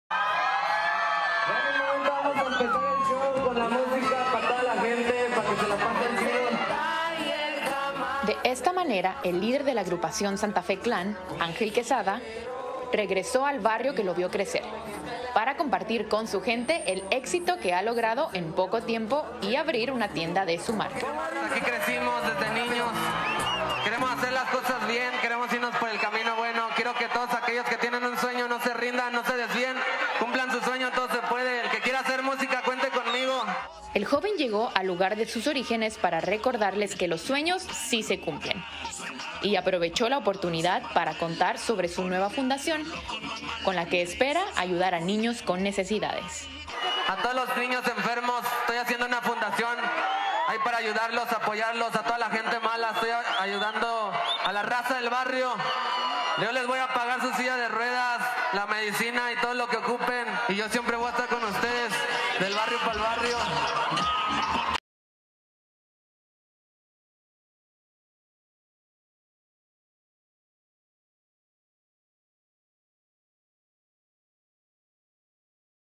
Santa Fe Klan regresó al barrio que lo vio nacer | Telemundo
Ángel Quezada, mejor conocido como Santa Fe Klan dio un concierto y abrió una tienda en el barrio mexicano que lo vio nacer, para recordarles que los sueños se cumplen.